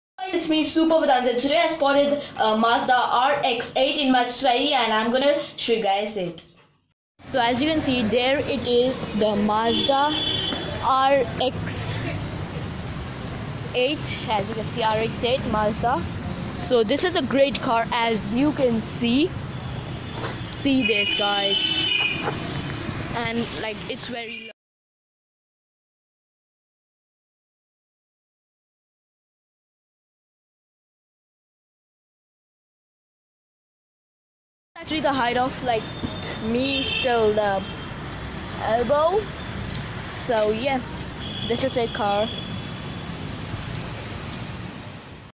Mazda RX8 | Car Spotted
This is a great classic looking car, Mazda RX8 is very low and not thar expensive car.